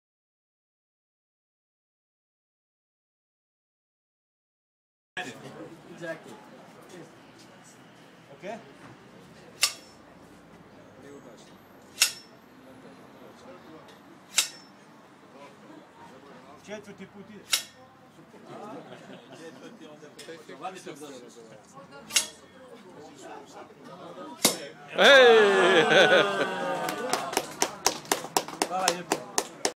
Zagreb Popping the Cork.wmv
Good demonstration of how to pop corks in Zagreb.